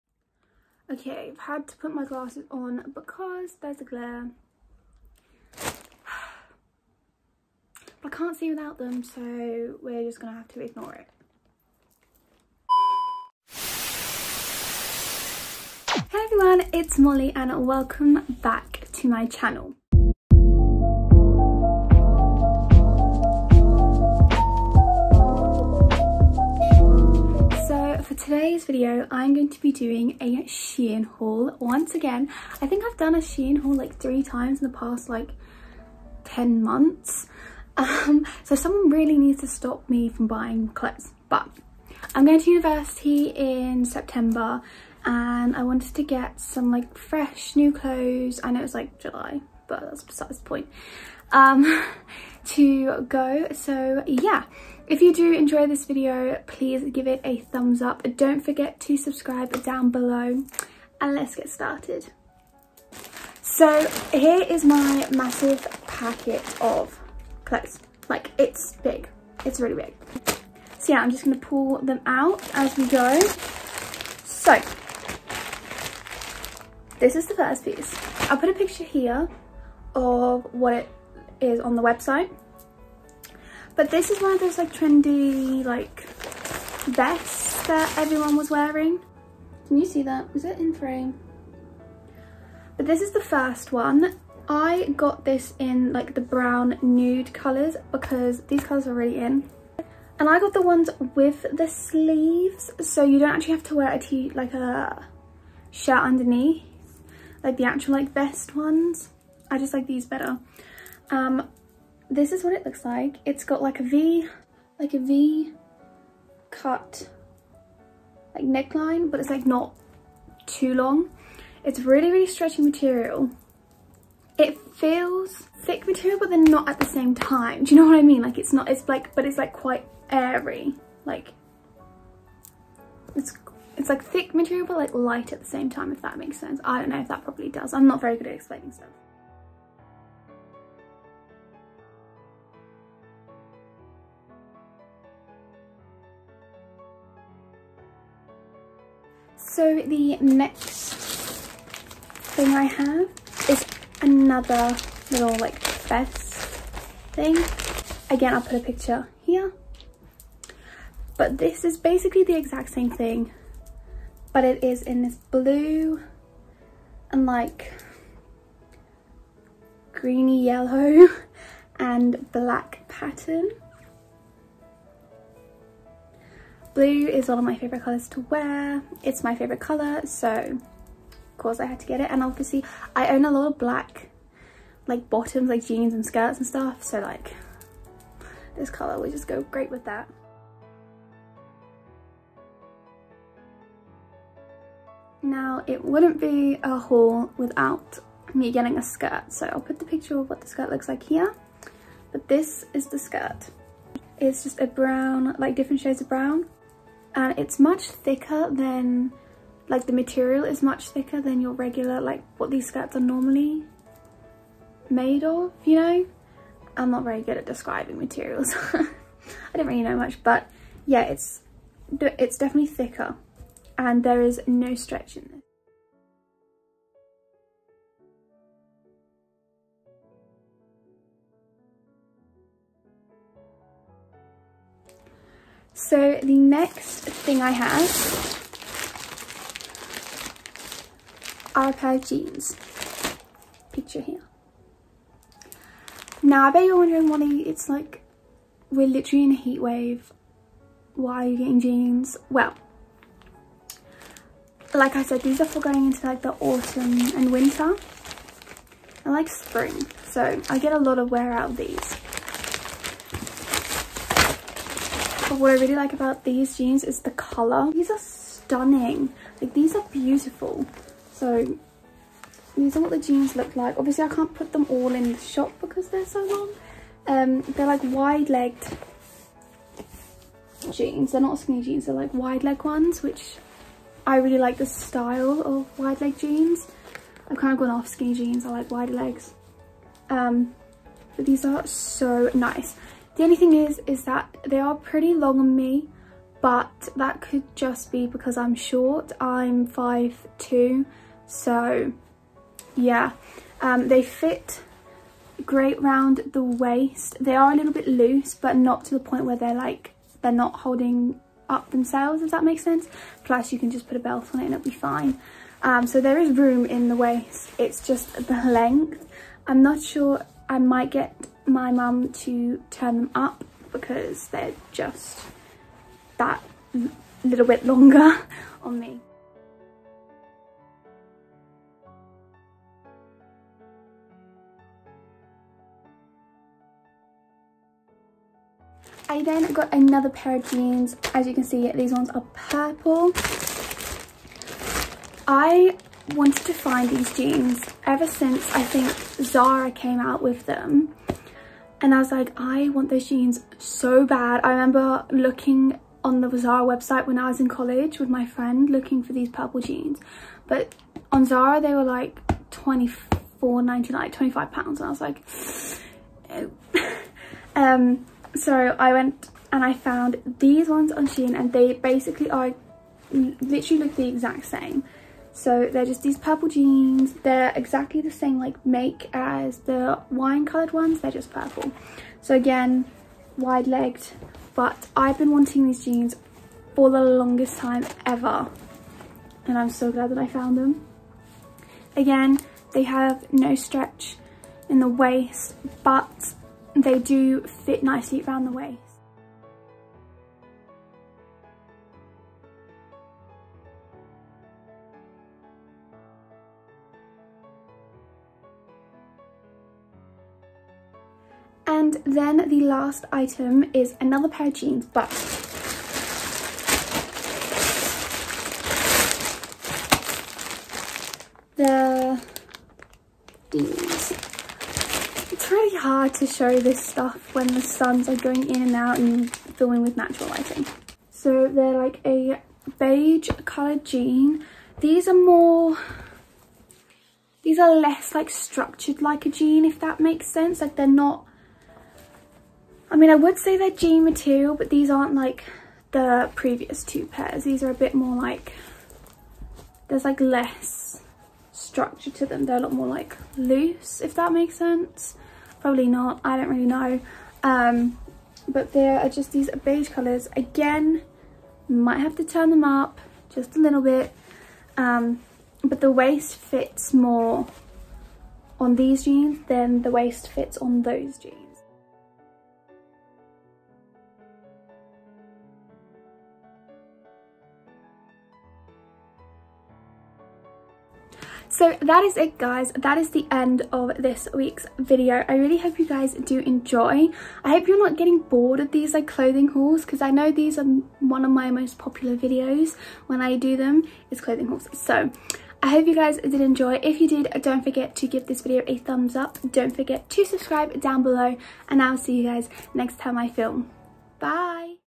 SHEIN *TRY ON* HAUL 😊
Hey guys,
So today’s video I ordered from shein and Wanted to show you all!